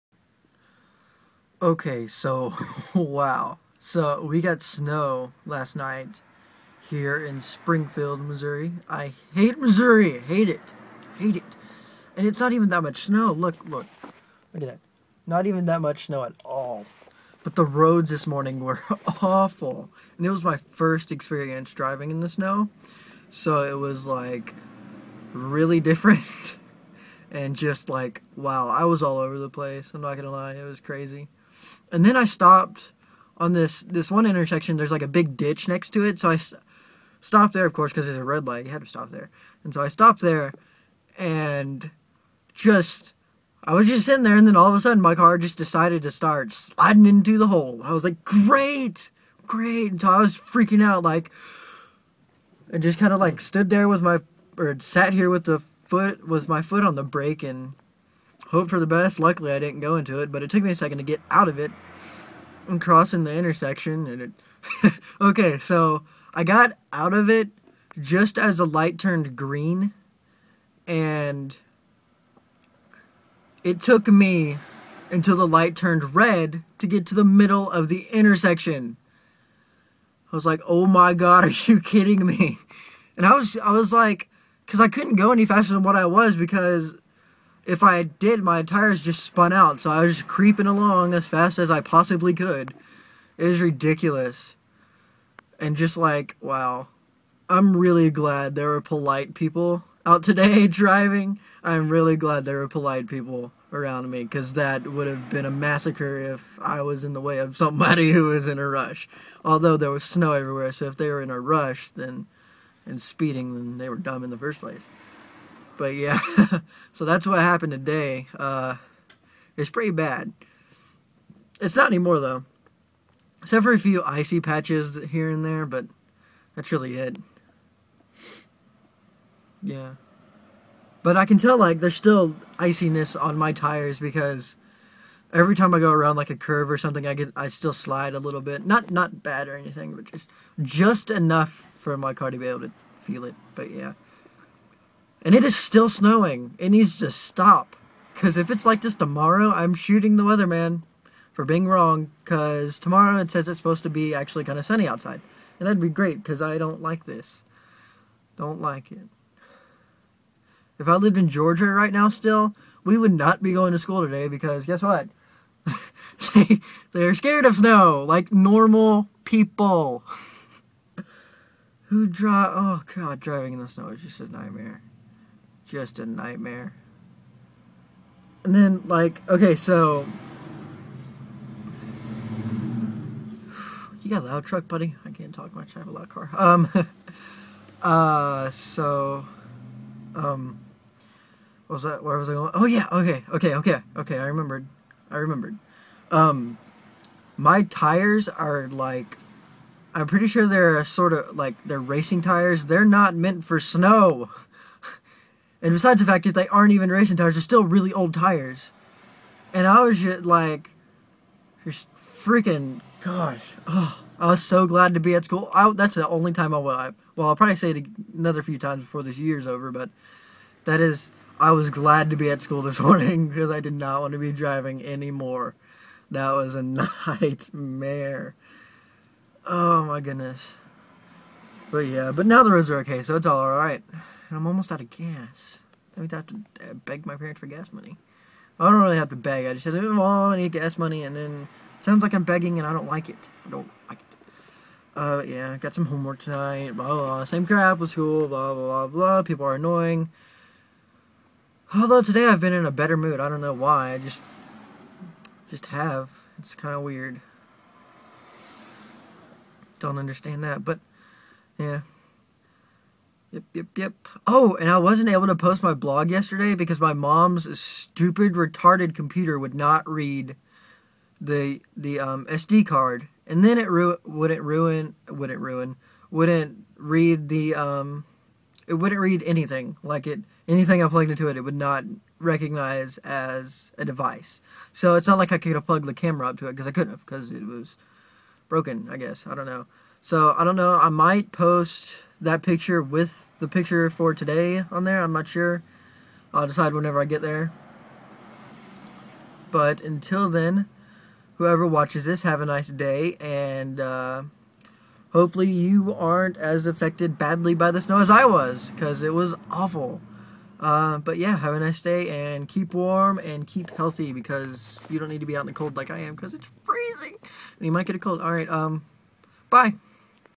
Sad Snowy Day
I pretty much talk about how much snow sucks! Haha